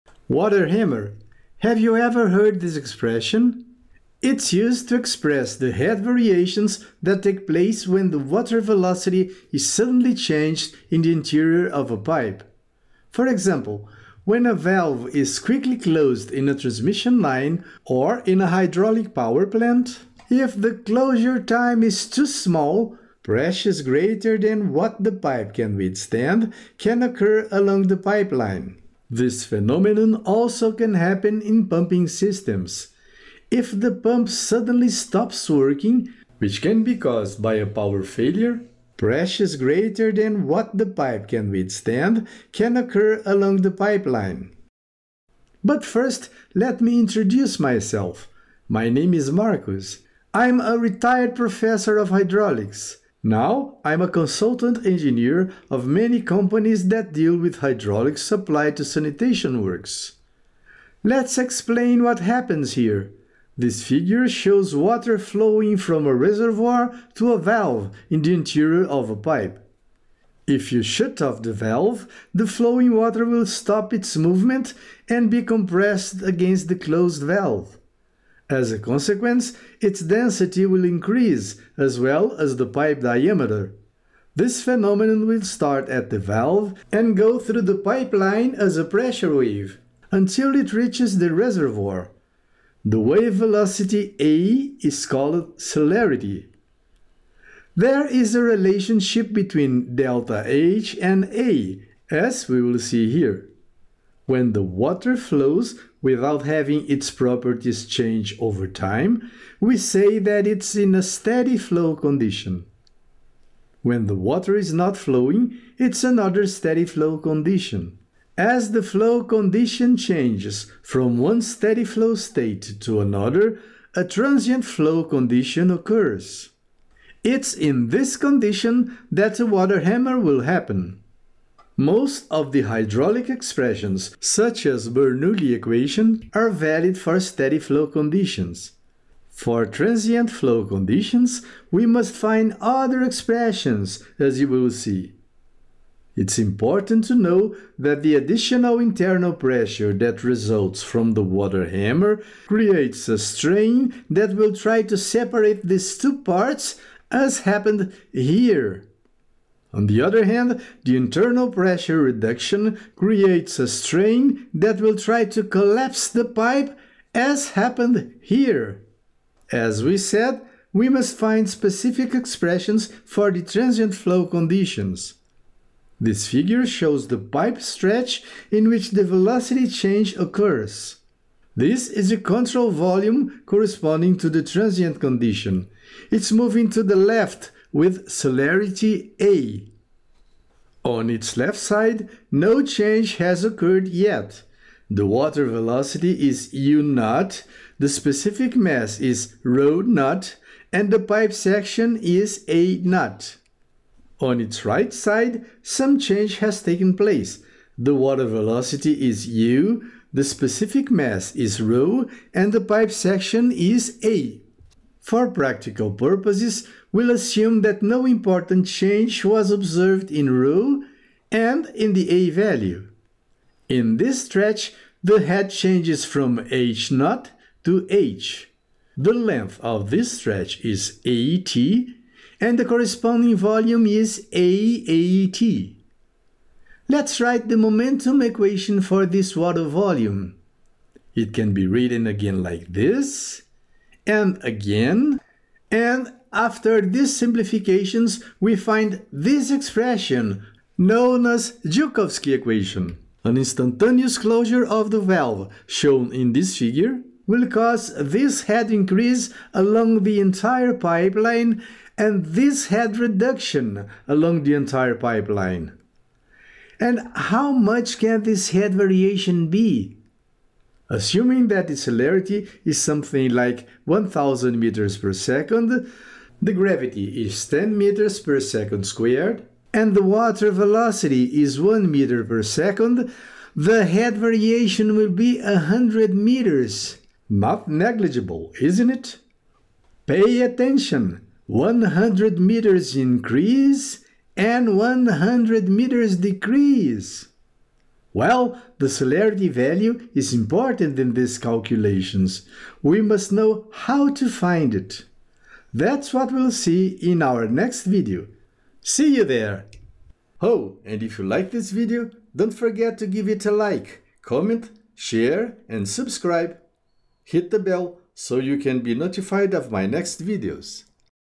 Water hammer: Joukowsky equation
In this video, Prof. Marcos Vianna presents the Joukowsky equation, which shows the relationship between head and water velocity variation in a pipeline.